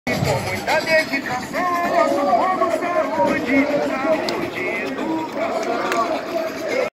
Eleições 2022: Bastidores da Convenção do PSDB em Campo Grande/MS VIDEOS 21
O evento político conteceu na sede do partido na sexta-feira 5 de agosto de 2022 em Campo Grande, Mato Grosso do Sul